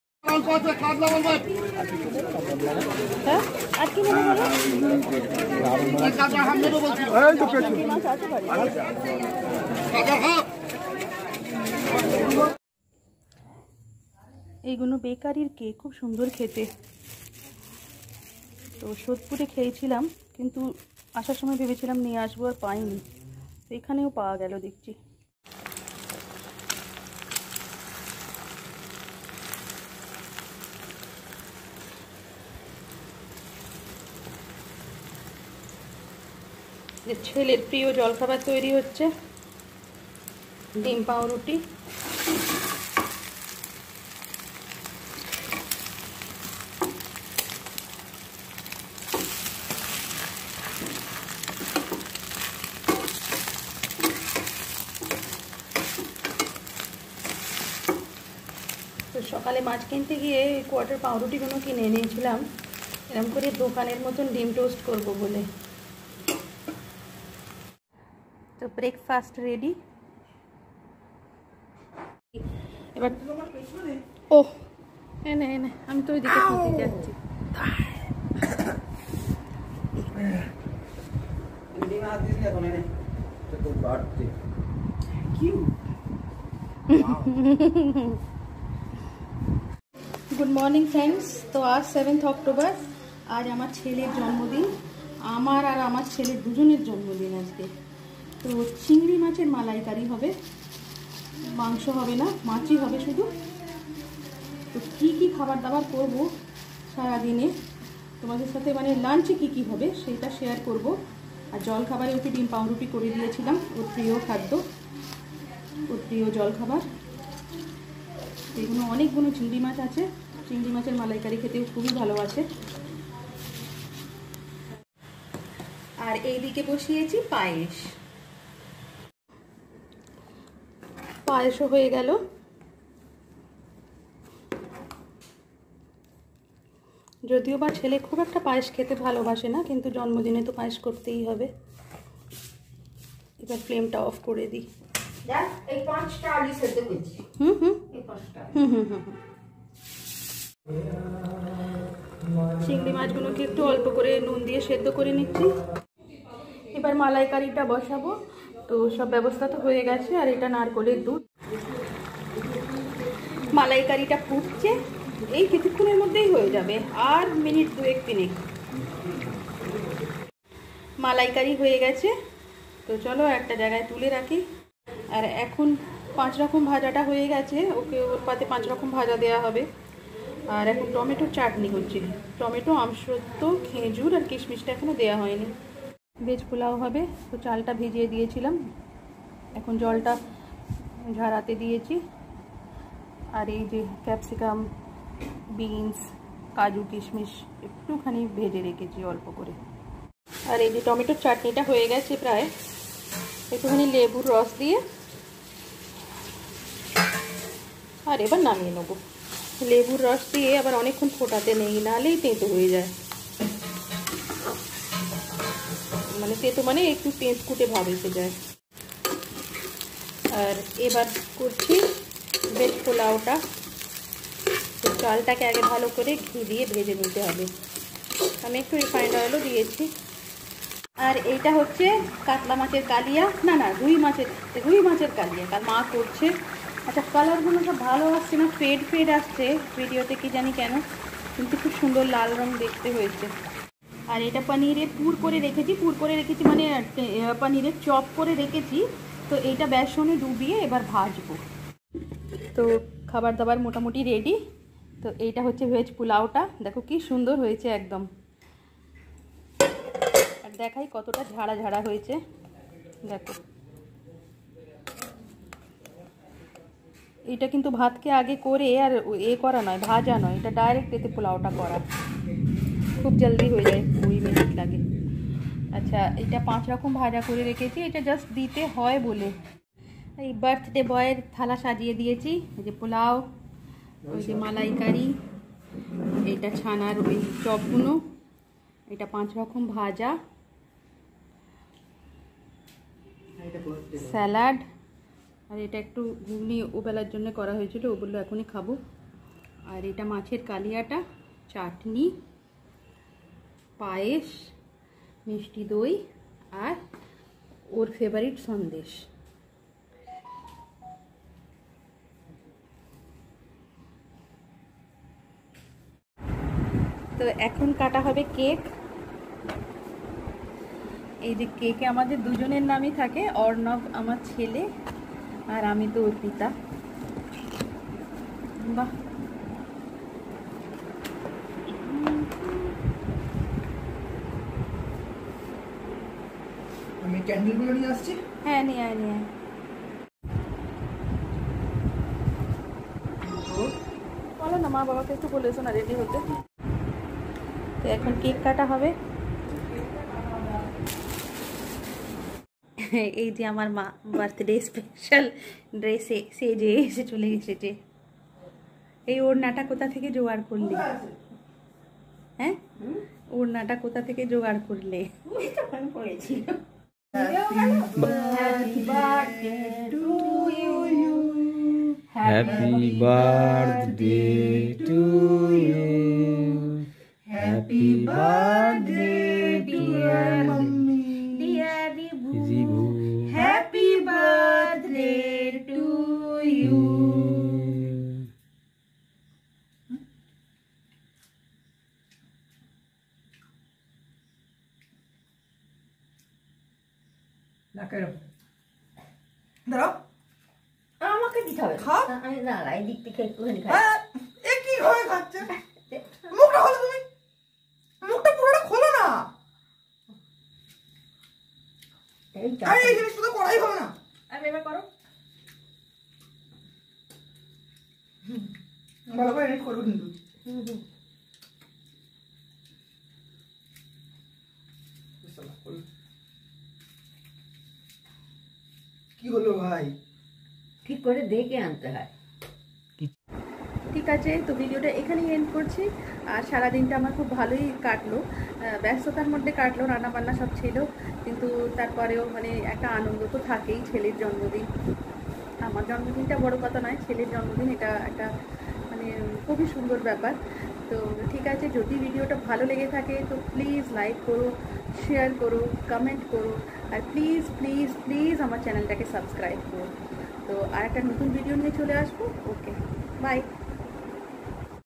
Cheler Jonmodeen/Sorry for very late upload | ARPITA's CANVAS
Cheler aar amr birthday celebration kore chilam choto kore...but upload korte deri hoe galo.

NAME : Zaika_Fusion